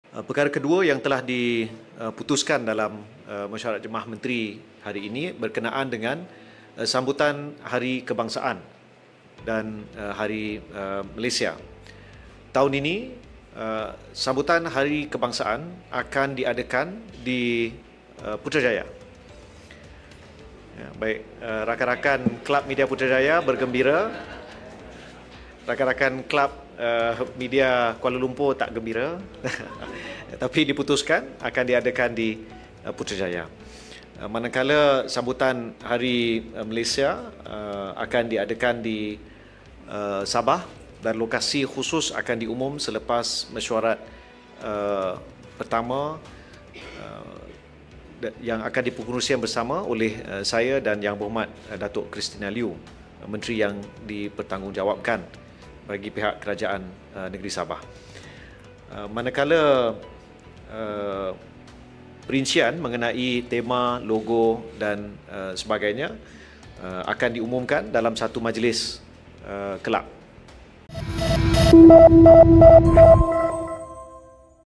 Putrajaya hos sambutan Hari Kebangsaan dua tahun berturut-turut
BERITA SEMASA 21 MEI 2024

Pusat Pentadbiran Kerajaan Persekutuan, Putrajaya dipilih buat kali kedua berturut-turut di bawah pemerintahan Kerajaan Madani untuk menjadi lokasi sambutan Hari Kebangsaan pada 31 Ogos nanti.

Menteri Komunikasi, Fahmi Fadzil memaklumkan perkara itu diputuskan di dalam mesyuarat Kabinet pada Selasa.

Menurutnya, Sabah pula dipilih sebagai lokasi sambutan Hari Malaysia pada 16 September.